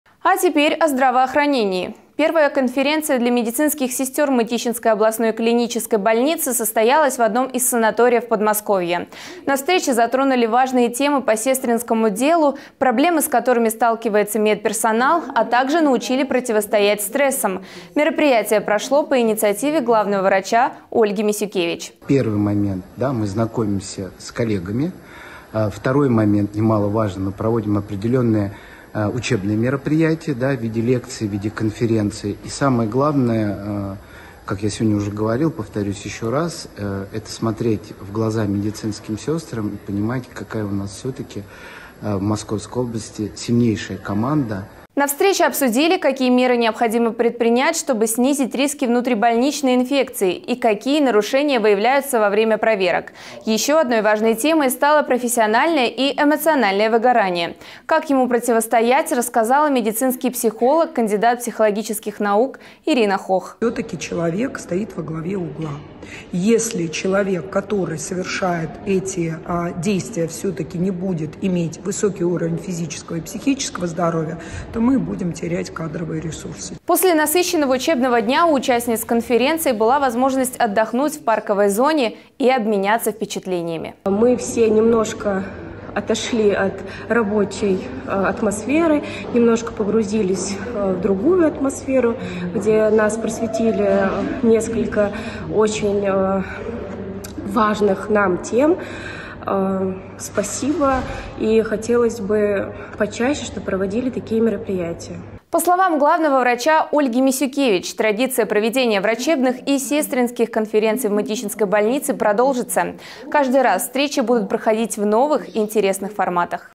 В Мытищинской больнице прошла первая конференция медицинских сестер
Первая конференция для медицинских сестер Мытищинской областной клинической больницы состоялась в одном из санаториев Подмосковья. На встрече затронули важные темы по сестринскому делу, проблемы с которым сталкивается медперсонал, а также научили противостоять стрессам. Мероприятие прошло по инициативе главного врача Ольги Мисюкевич. (12+)